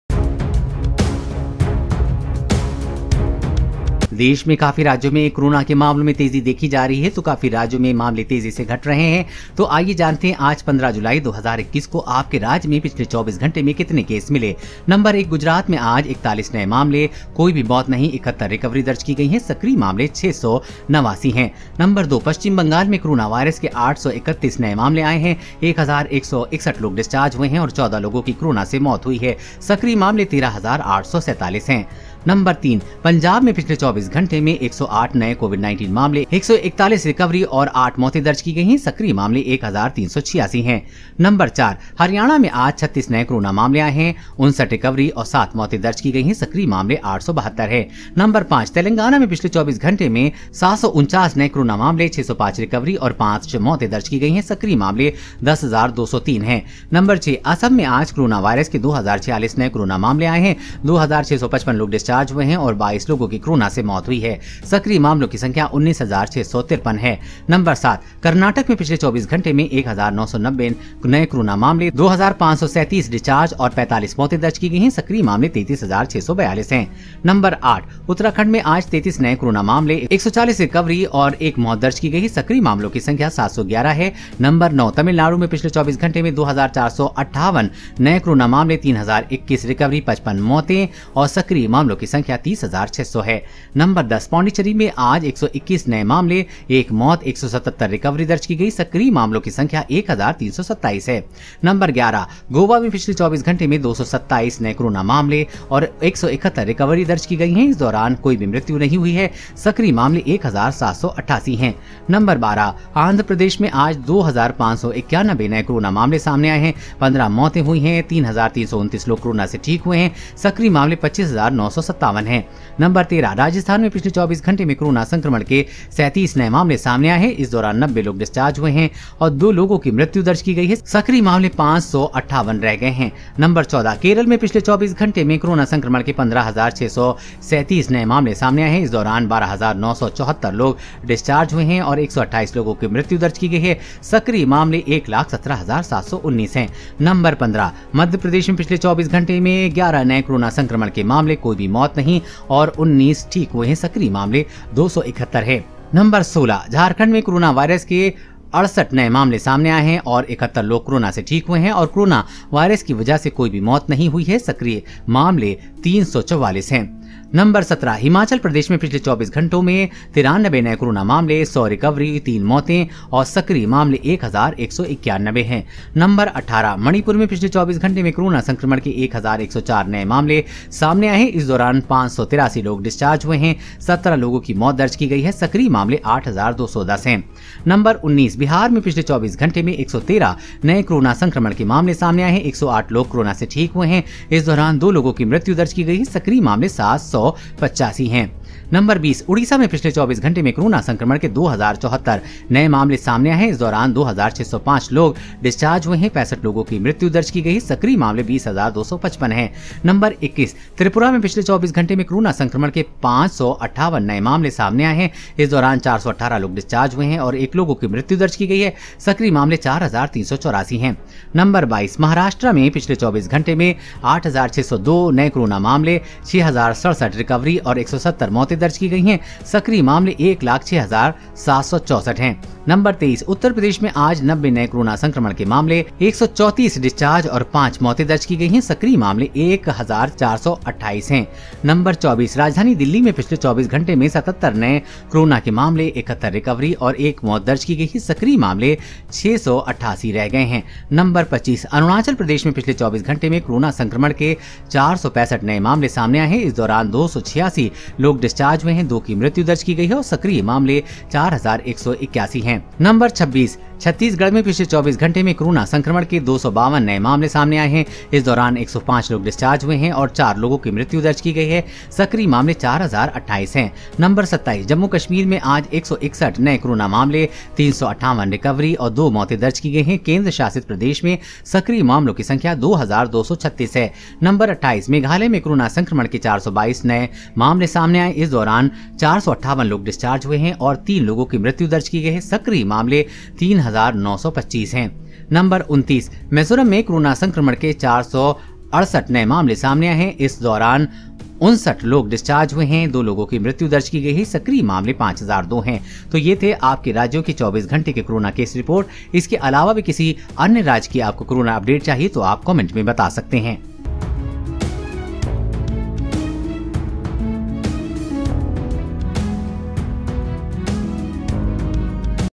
Coronavirus : जानिए किस राज्य में आये कितने केस, आपके राज्य में कोरोना का हाल !! 15 जुलाई 2021
Corona Update: किस राज्य में आज कितने नए केस ? जानिए सभी राज्यों की कोरोना केस रिपोर्ट ! 15 जुलाई 2021

Coronavirus (COVID-19) Cases In India State Wise Daily Updates | Coronavirus Cases State Updates | All State Coronavirus Case Update